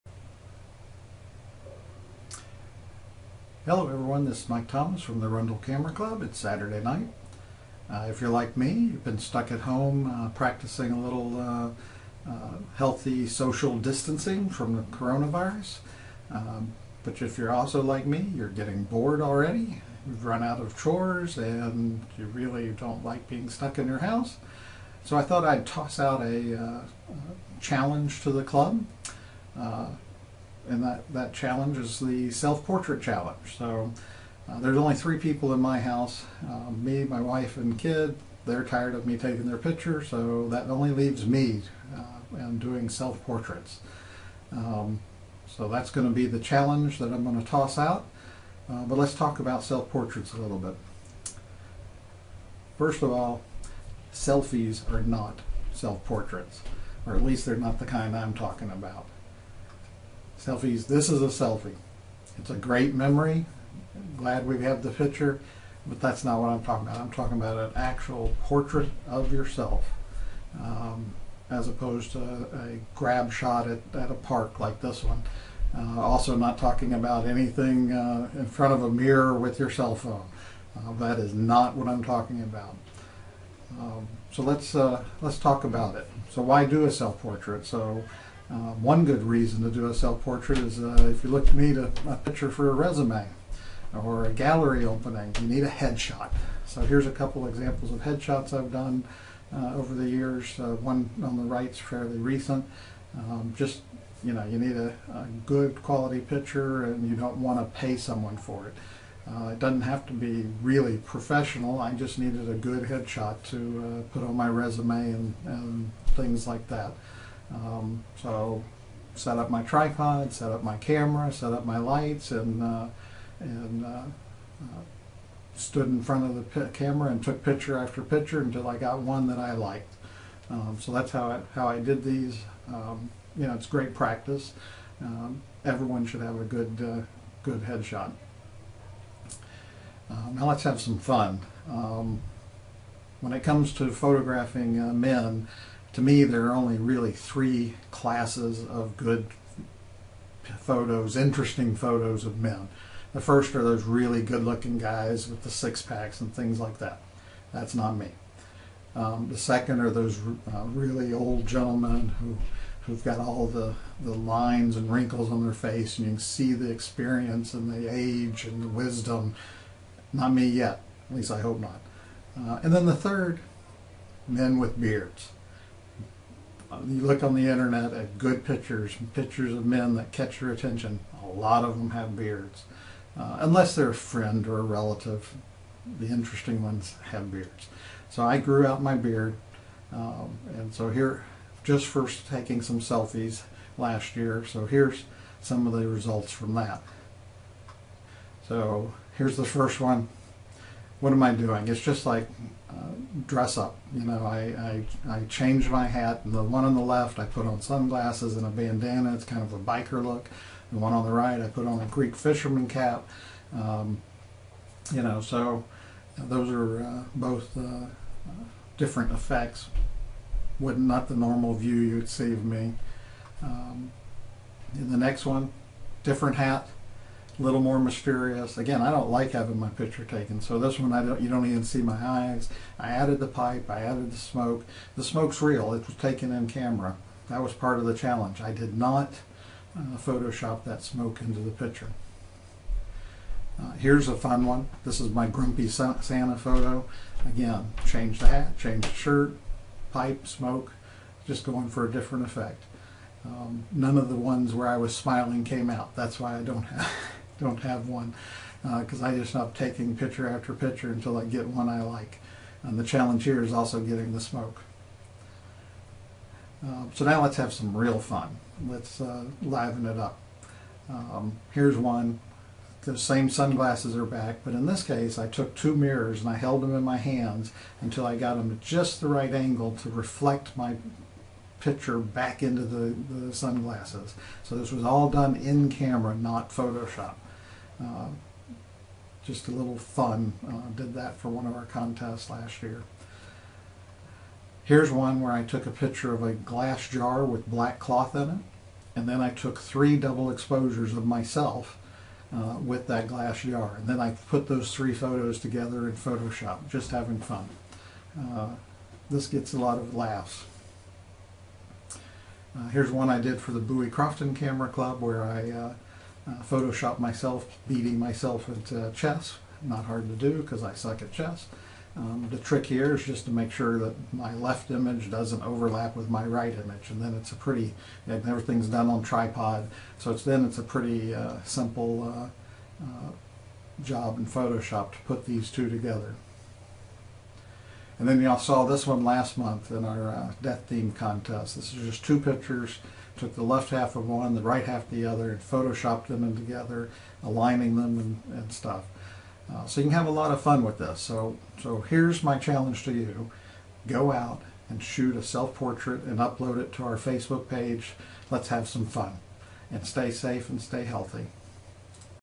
ACC: Photo Assignment - Self Portrait - Mike Thomas, March 25, 2020
A challenge to the Arundel Camera Club to shoot a self portrait during the COVID-19 social distancing.